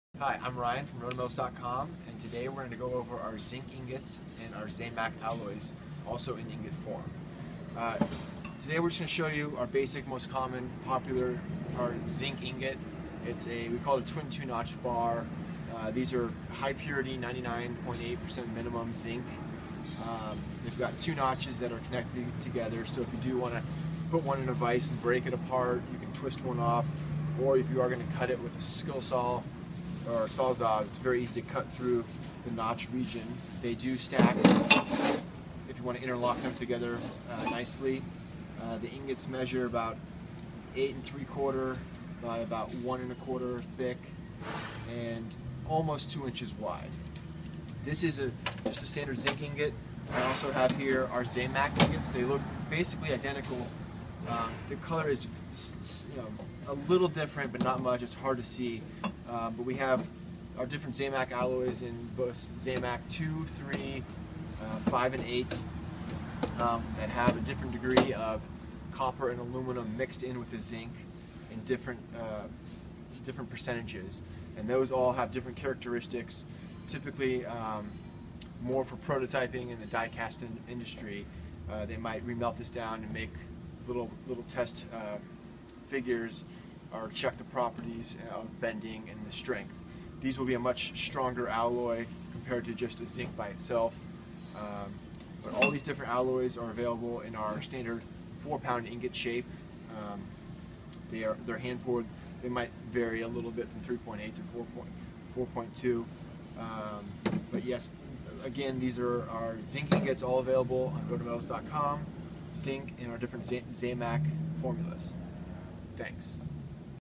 RotoMetals.com Zinc Ingots and Zamak Alloys
Shows how the different Zinc notched ingots and ZAMAK alloys for die casting.